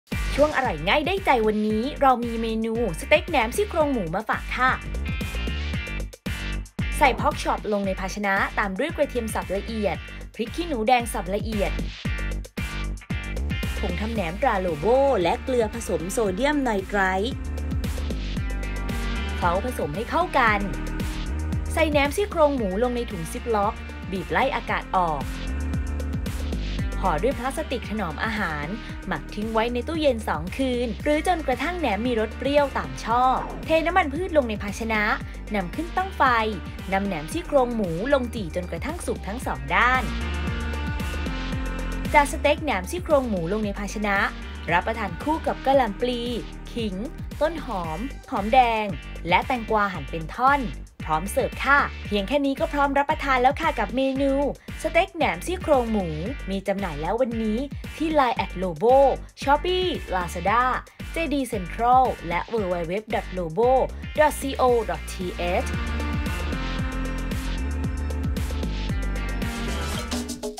สเต๊กแหนมซี่โครงหมู ทำง่าย สไตล์ "อ.ยิ่งศักดิ์"
สเต็กแหนมซี่โครงหมู (ผลิตภัณฑ์  ผงทำแหนม ตรา โลโบ)

พอร์คชอป (ชิ้นละ 250  กรัม) 2 ชิ้น
กระเทียมสับละเอียด   2  ช้อนโต๊ะ
พริกขี้หนูแดงสับละเอียด   1  ช้อนโต๊ะ
ผงทำแหนม ตราโลโบ   35  กรัม
น้ำมันพืช สำหรับจี่
กะหล่ำปลีแกะใบไม้ ขิงแกะดอกไม้ ต้นหอม พริกแกะดอกไม้ หอมแดงแกะดอกไม้และแตงกวาหั่นเป็นท่อน สำหรับรับประทานคู่

1.ใส่พอร์คชอปลงในภาชนะ ตามด้วยกระเทียมสับละเอียด พริกขี้หนูแดงสับละเอียด และผงทำแหนม ตราโลโบ เคล้าผสมให้เข้ากัน
2.ใส่แหนมซี่โครงหมูลงในถุงซิปล็อก บีบไล่อากาศออก ห่อด้วยพลาสติกถนอมอาหาร หมักทิ้งไว้ในตู้เย็น 2 คืน หรือจนกระทั่งแหนมมีรสเปรี้ยวตามชอบ
3. เทน้ำมันพืชลงในภาชนะ นำขึ้นตั้งไฟ นำแหนมซี่โครงหมูลงจี่จนกระทั่งสุกทั้งสองด้าน
4.จัดสเต็กแหนมซี่โครงหมูลงในภาชนะ รับประทานคู่กับกะหล่ำปลีแกะใบไม้ ขิงแกะดอกไม้ ต้นหอม พริกแกะดอกไม้ หอมแดงแกะดอกไม้และแตงกวาหั่นเป็นท่อน พร้อมเสิร์ฟ

▲Line @yingsakfood